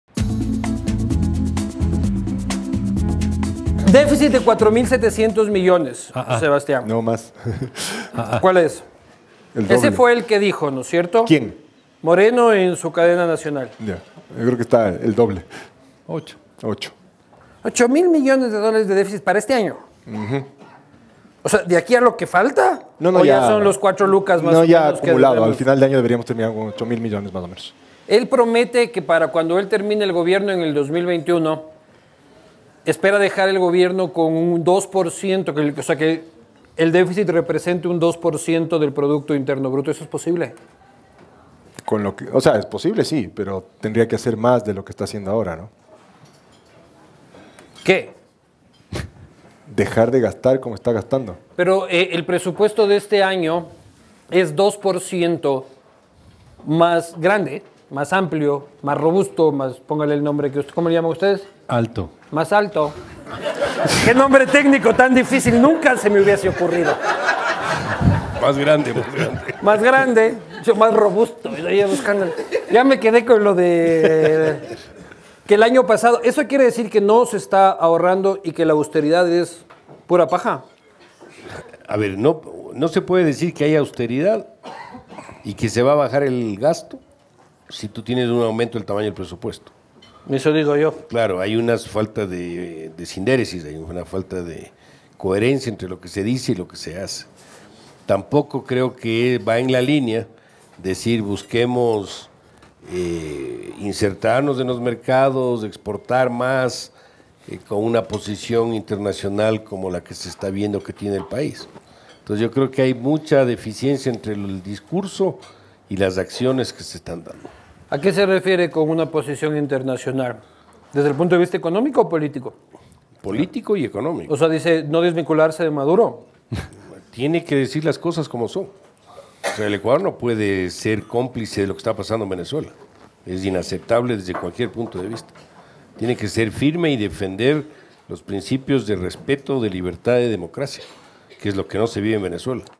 Clip1 Presupuesto más alto
Los tres economistas hablan de cuánta plata mismo nos vamos a gastar.